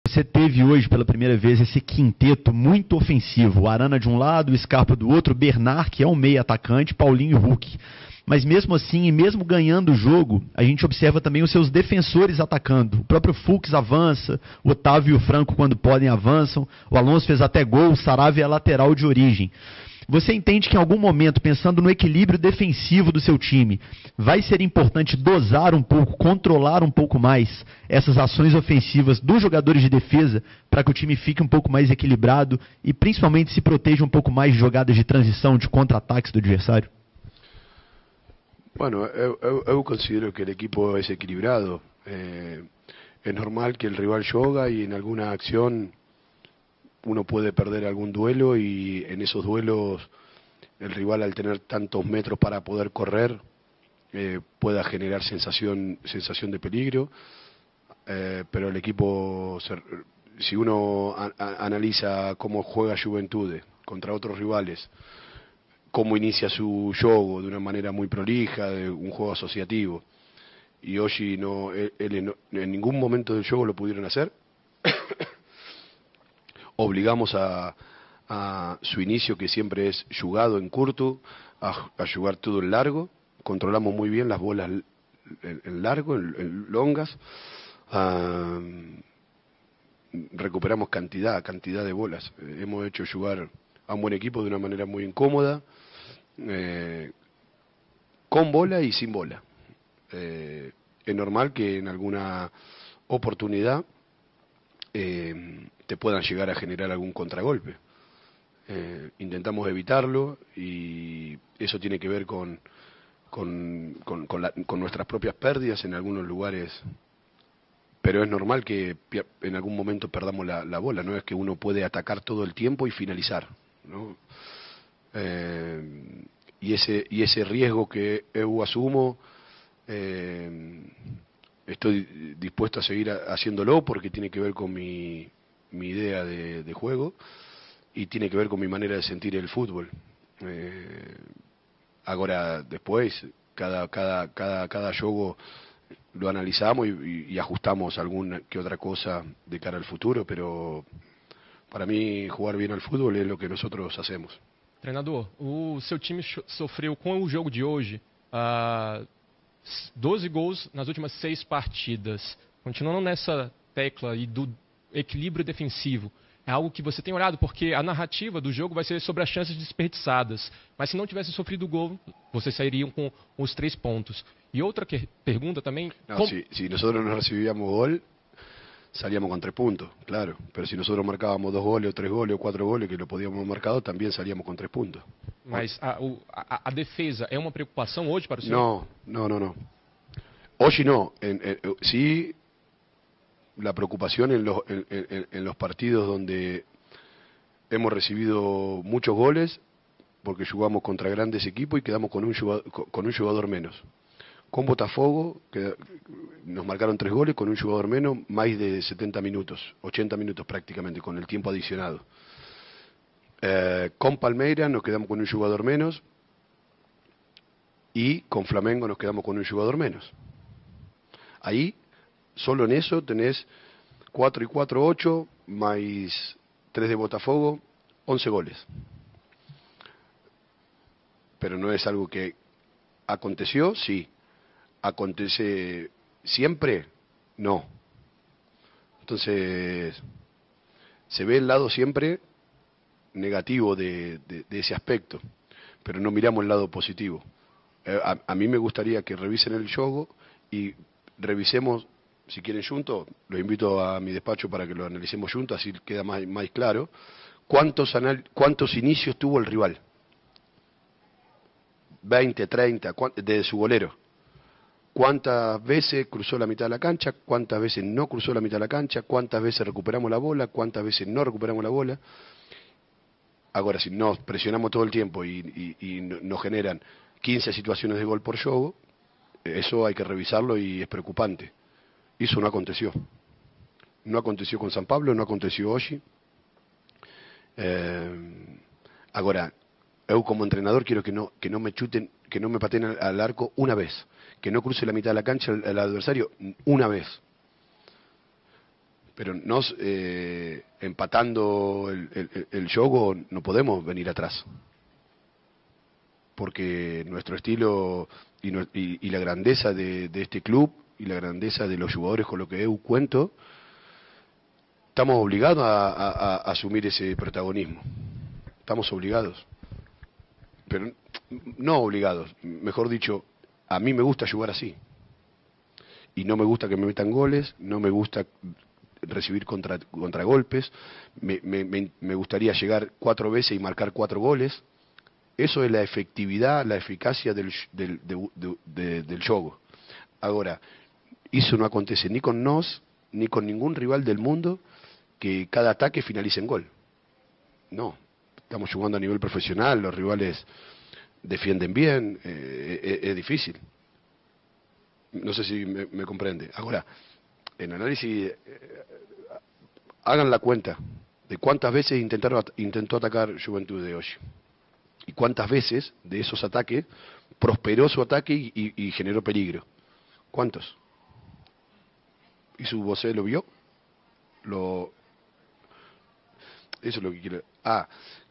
2ª parte Coletiva Milito técnico do Atlético no empate com Juventude
Milito fala da estreia do Bernard e Junior Alonso e a qualidade dos jogadores.